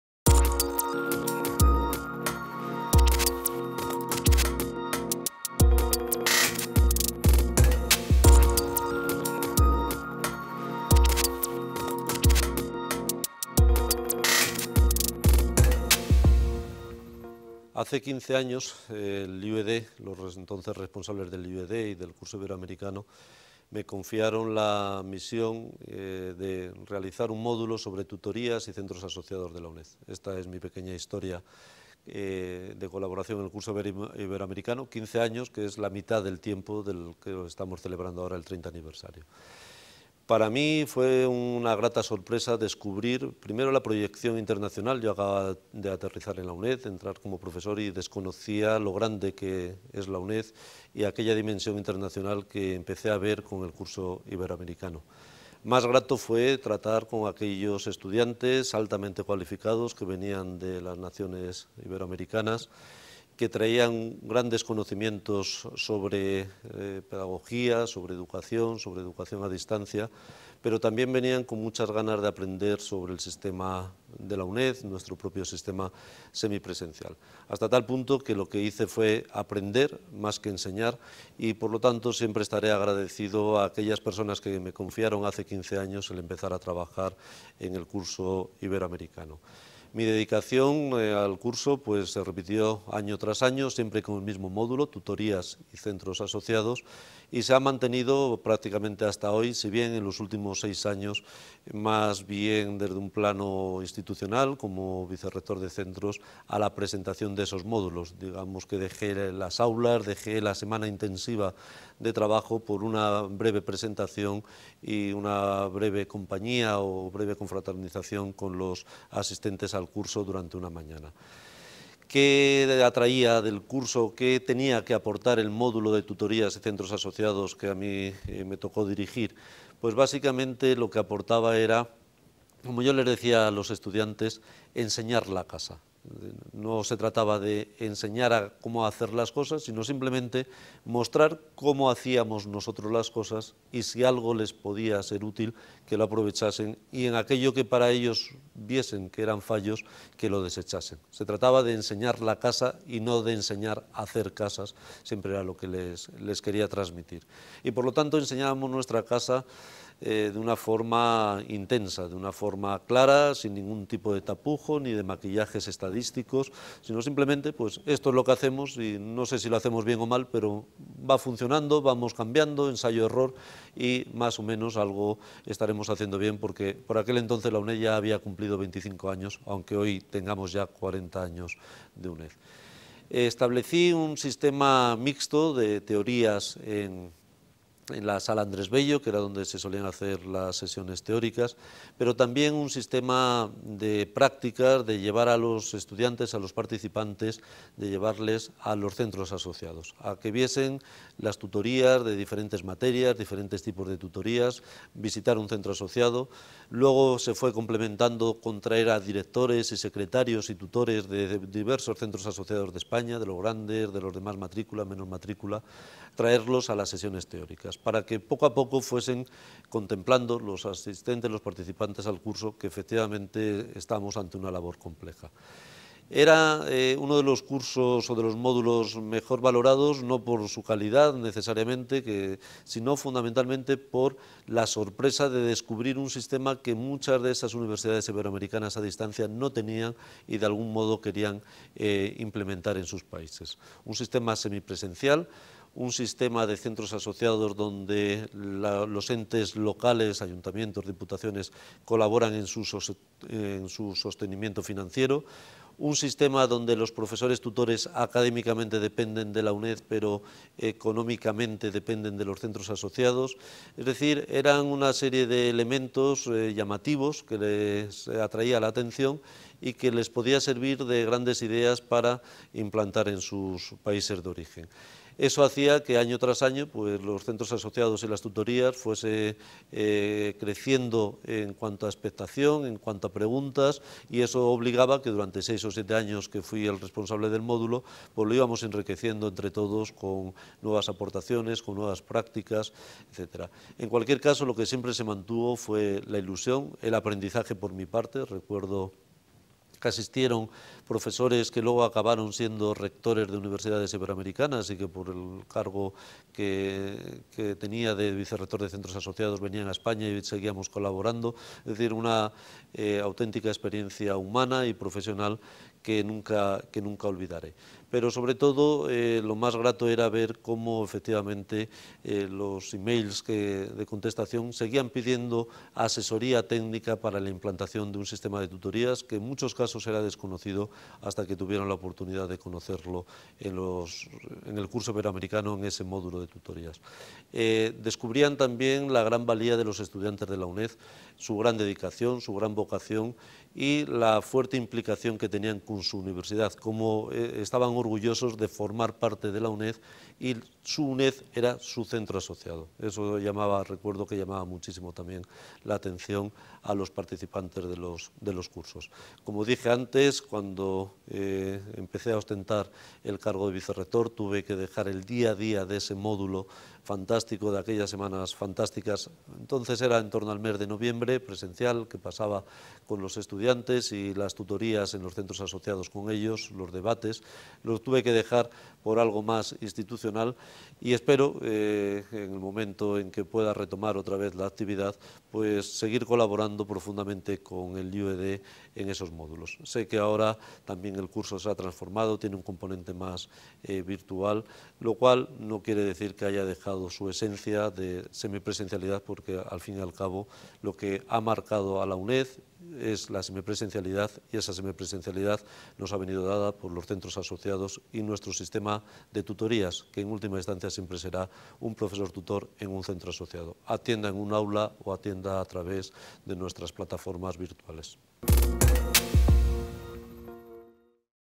XXX CIED - Los Centros Asociados. Antonio Fernández Fernández
Los Centros Asociados.

XXX CIED - 30 Aniversario del curso Iberoamericano de Educación a Distanica.

XXX Curso Iberoamericano de Educación a Distancia (CIED)

El Curso Iberoamericano de Educación a Distancia (CIED) de la UNED ha cumplido 30 años. Este aniversario ha coincidido con la celebración de los 40 años de la UNED.

Este curso es el programa de cooperación al desarrollo de más larga tradición en la UNED y está destinado a la formación de profesionales de la educación (docentes, gestores públicos y privados) cuyas instituciones y/o países, estén interesados en la implementación o mejora de sistemas de educación a distancia.

A lo largo de 29 ediciones, casi 700 profesionales de 17 países han cursado este programa que, durante sus 21 primeras ediciones tuvo un formato presencial y, a partir de la edición 22 un formato semipresencial, con especial énfasis en el trabajo en comunidad virtual de aprendizaje a lo largo de siete meses.